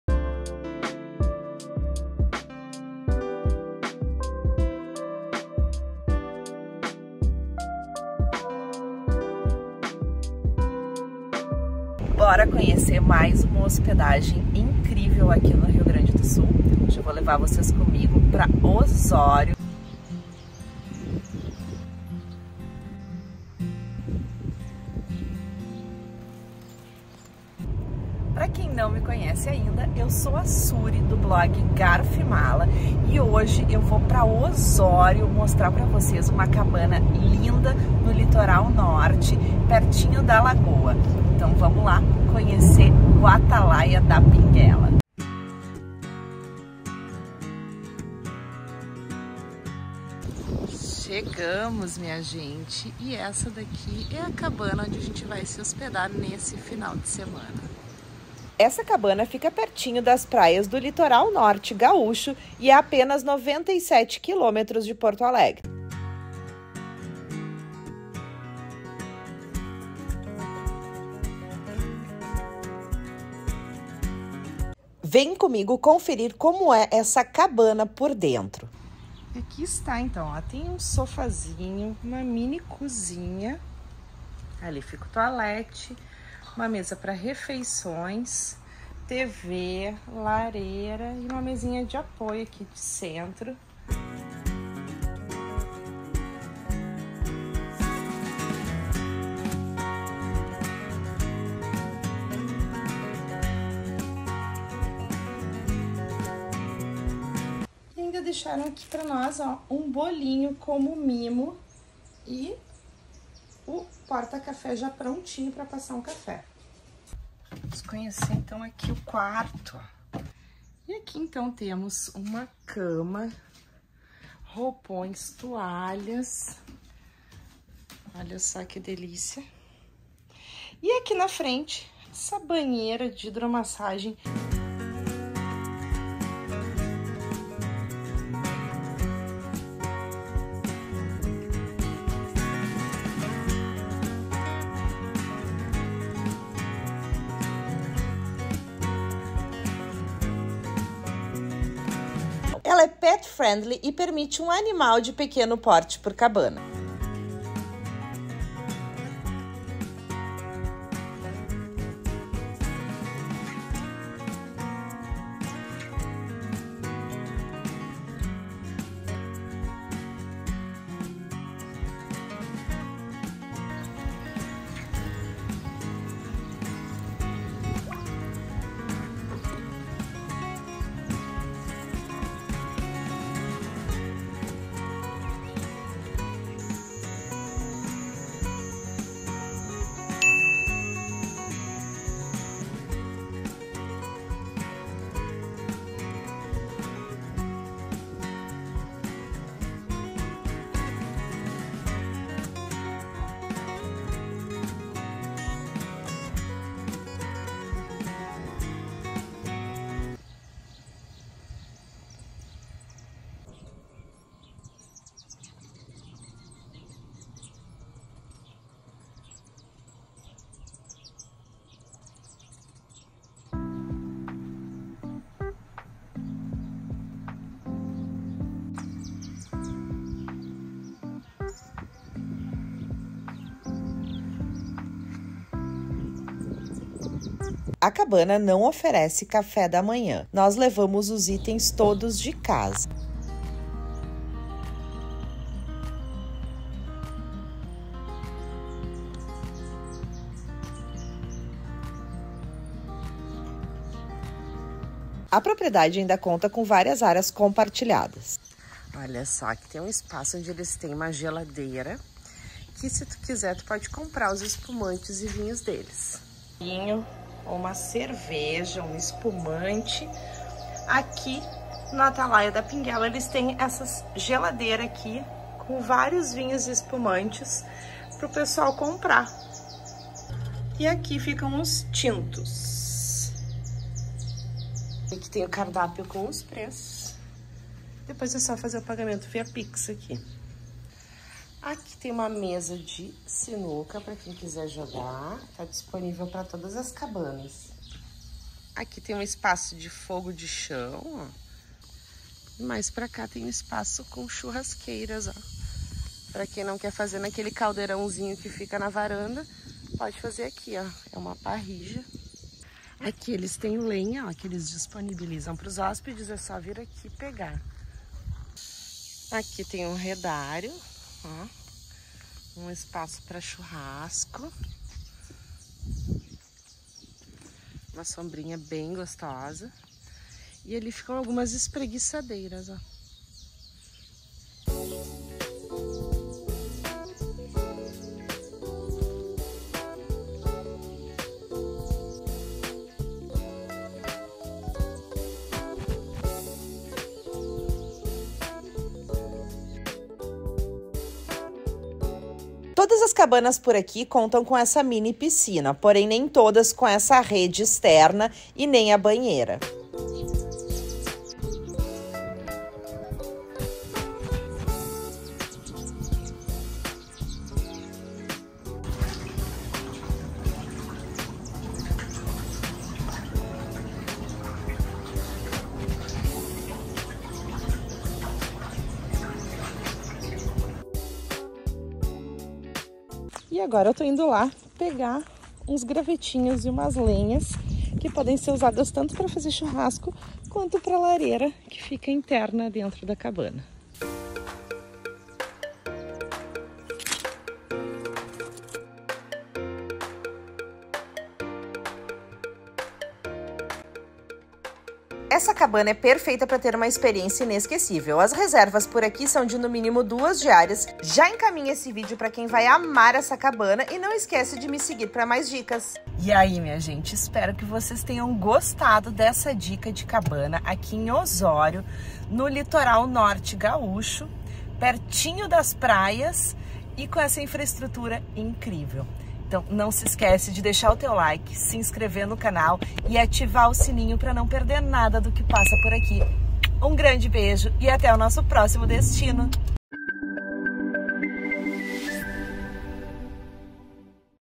CABANA EM OSÓRIO - RS | Vem ver como é essa tiny house no litoral norte gaúcho!
▶️ Aperta o PLAY e vem conferir tudo sobre essa cabana tiny house em Osório no RS!

Vem ver como é a cabana e sua infraestrutura no litoral norte gaúcho. Uma dica incrível para quem quer relaxar e desconectar, com direito a mini piscina e banheira!!!

Me conta o que achou deste vídeo! Já compartilha com quem vai amar essa hospedagem em Osório!

Até a próximo meu povo!